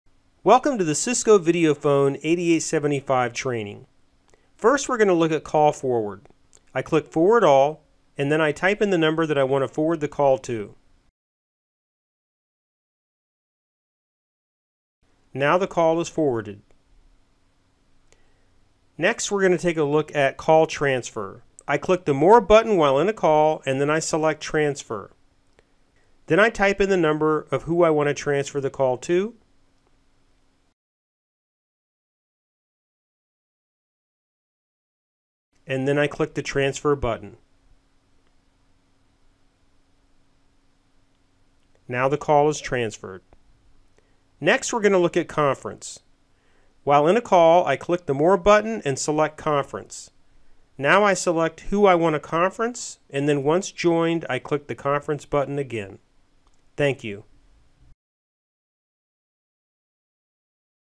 Cisco Video Phone 8875 Forward Transfer and Conference
Learn how to perform the common call tasks such as call forward, transfer, and conference on Video Phone 8875.